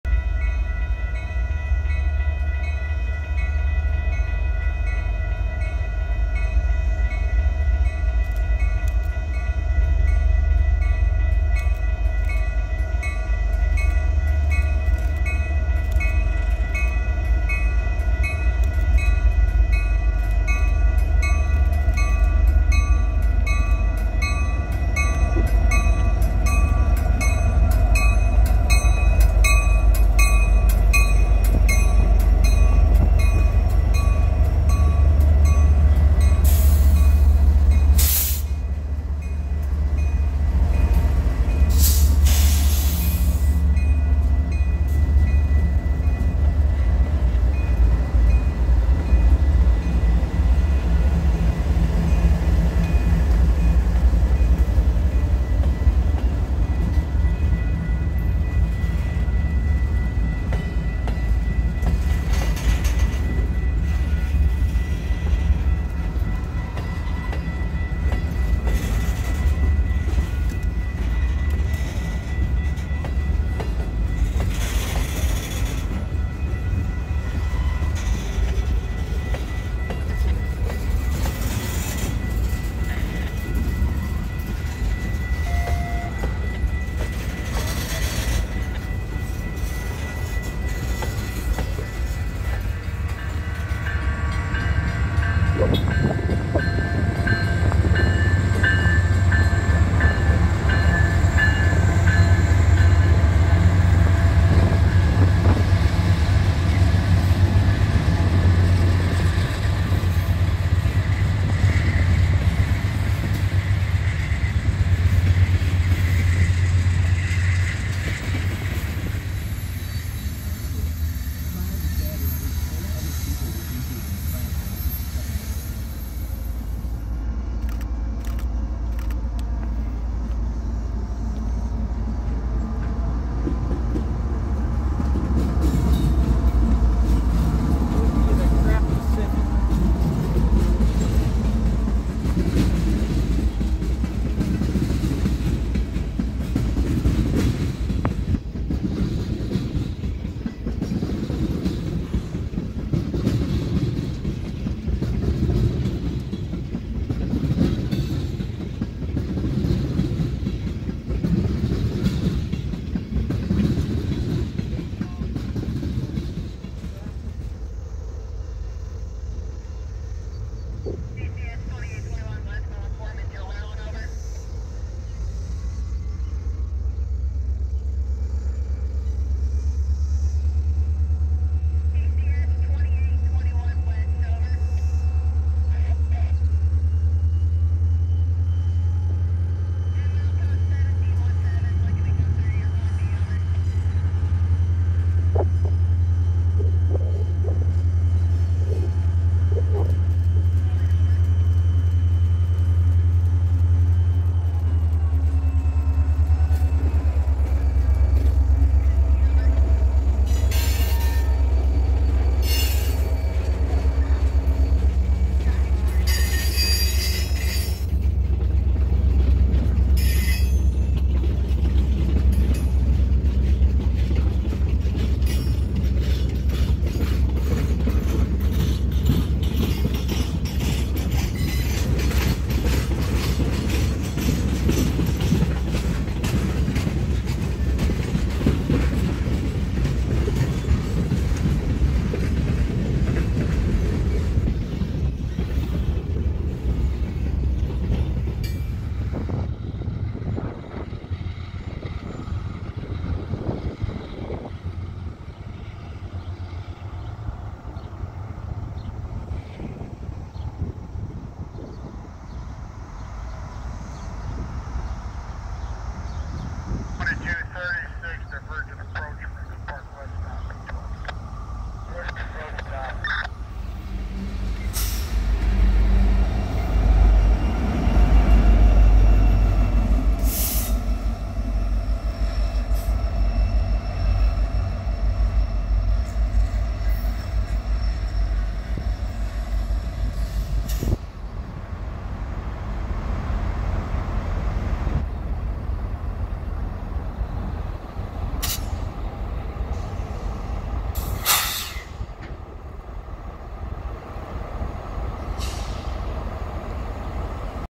KCS 2821 leads weed spraying train out of Bensenville, plus other CPKC action!
I ventured to Franklin Park on a limb that the annual weed spraying train would hopefully go to A5 for a spin and return west. Needless to say they ran around the train and headed west as is. While not an ideal situation, I still got the train and some other action while goobing around town.
CPKC 231: KCSM 4072, BNSF 7069, BNSF 707, BNSF 7627
547: CP 8207, KCSM 4567
528: CP 8949, CP 8945, rear KCS 4113 (later in the vid)
Weed spraying train: KCS 2821
6-4-24